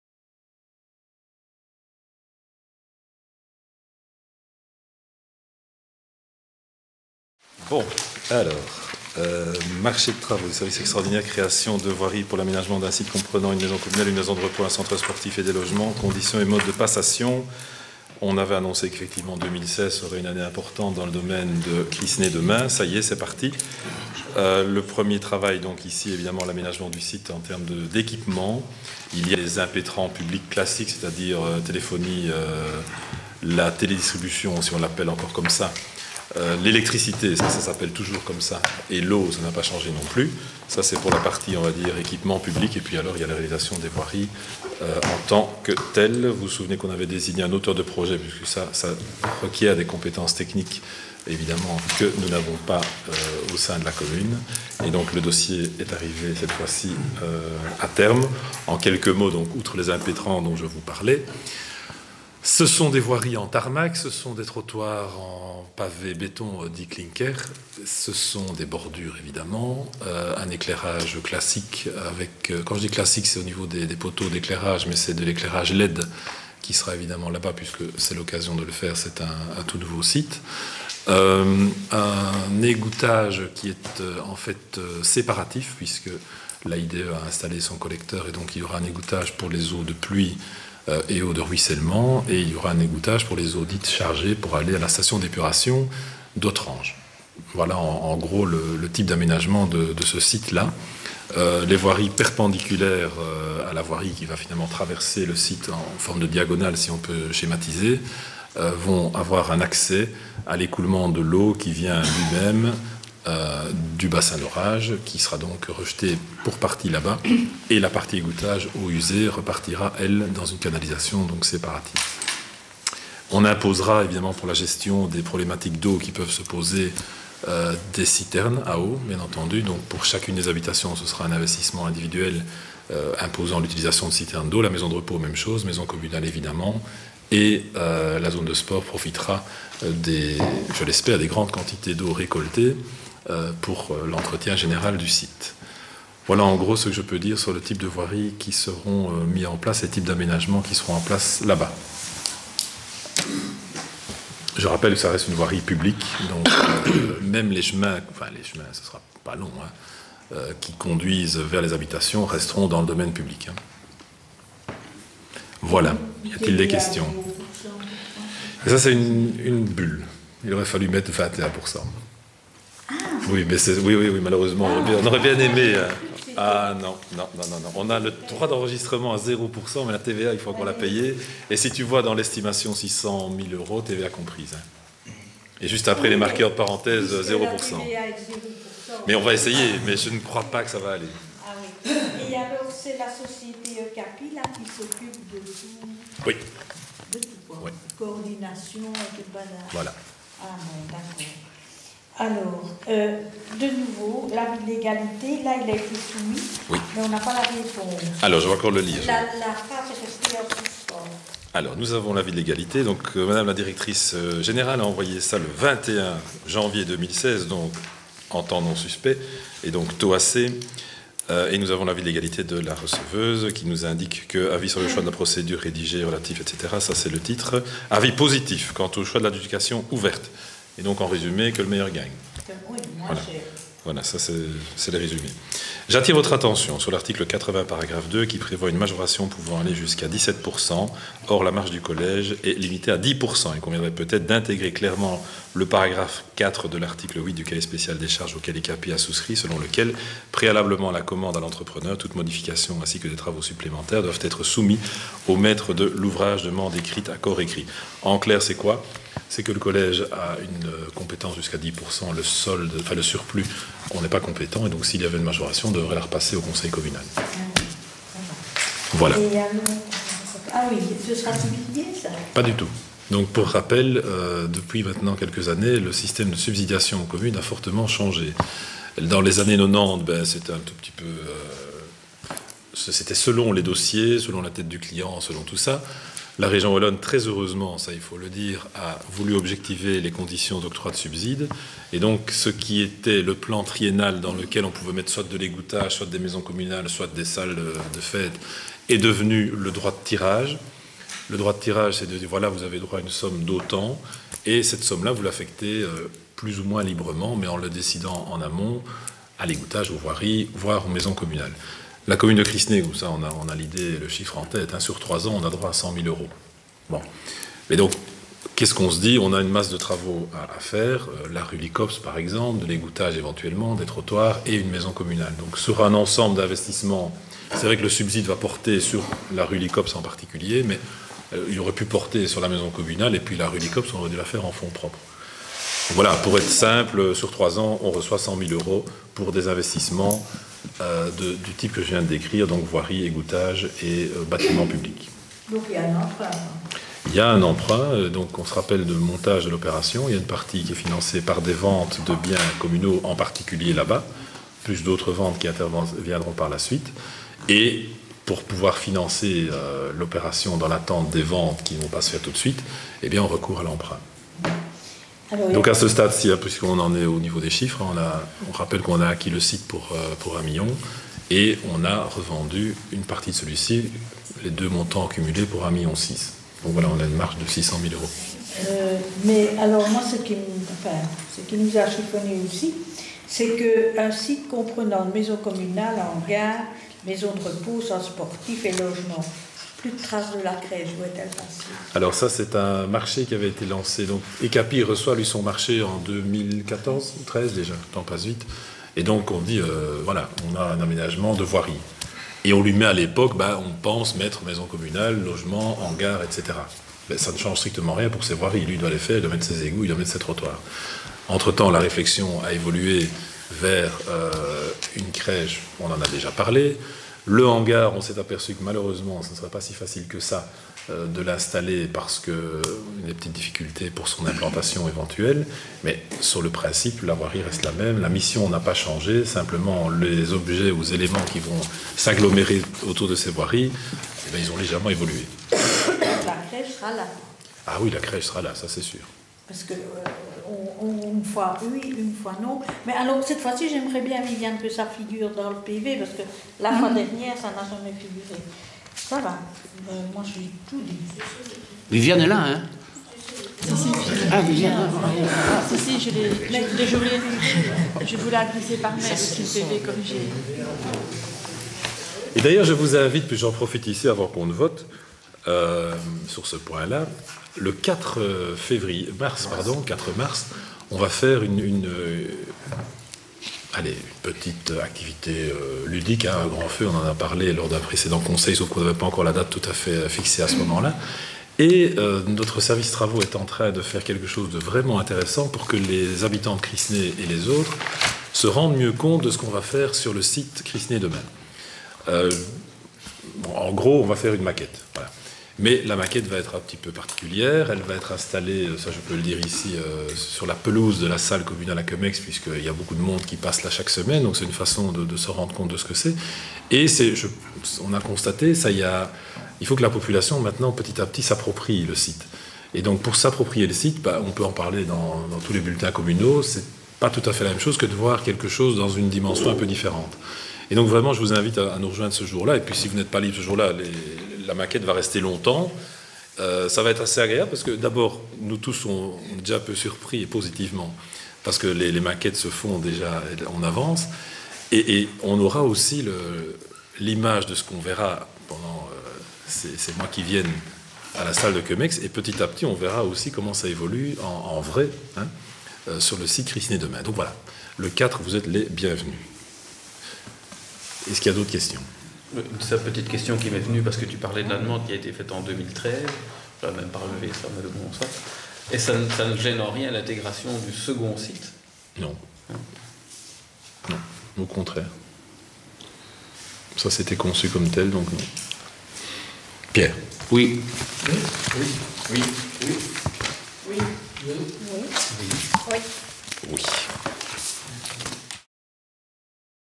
Conseil communal de Crisnée du 3 février 2016 point 7
Marché public de travaux du service extraordinaire - Création de voirie pour l'aménagement d'un site comprenant une maison communale, une maison de repos, un centre sportif et des logements - Conditions et  mode de passation.